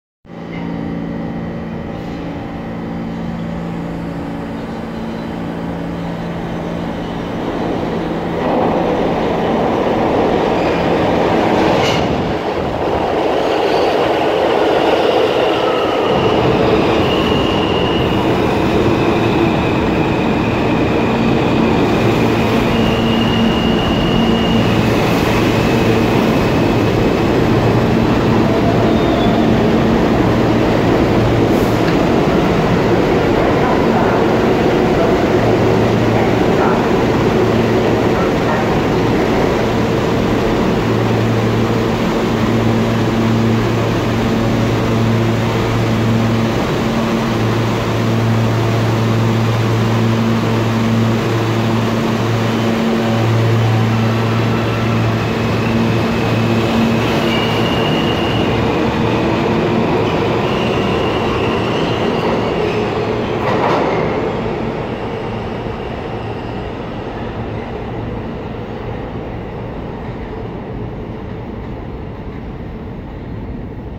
IND Fulton Street Line: 168th Street Bound R160A-1 (C) Train @ Utica Avenue
Seen here is R160A-1 (C) Train @ Utica Avenue heading towards 168th Street Manhattan. Due to the Bombardier R179s having issues once again, all of them have been taken OOS for inspection. Its temporary replacements will be the Budd Company R32s & Alstom R160A-1.

Bus Transfers: B25, B46, B46 SBS.

Train Transfers: (A) Train.